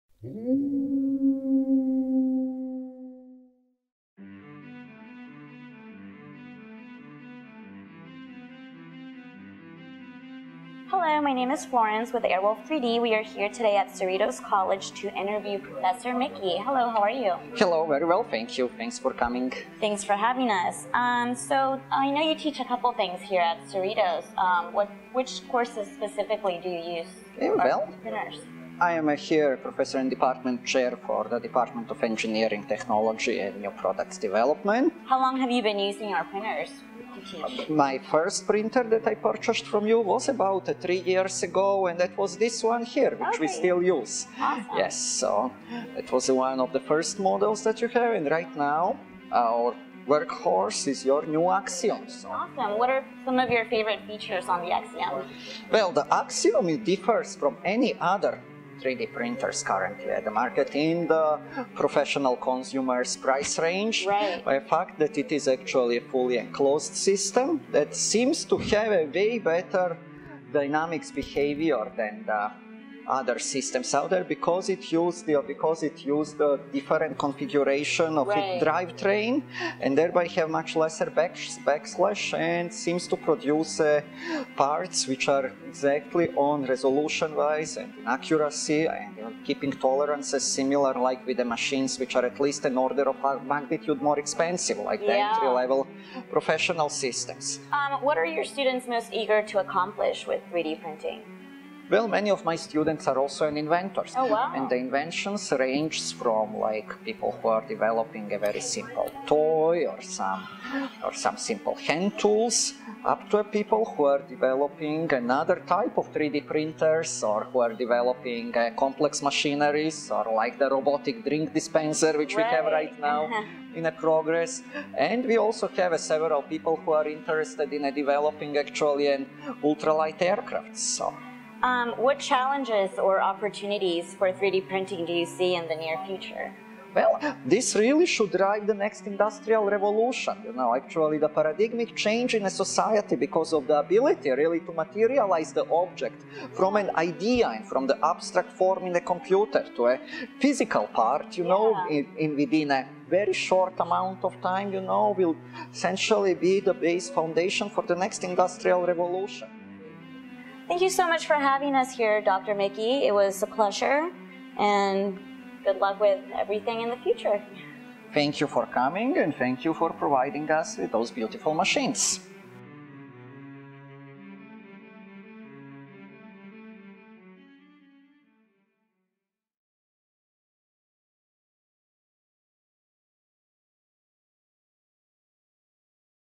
Airwolf 3D Interviews DR. Miodrag Micic
Airwolf 3D’s exclusive interview with Dr. Miodrag Micic. Learn how Dr. Miodrag Micic teaches students at Cerritos College the use of 3D technology and how to materialize their creations in the department of Engineering and Technology.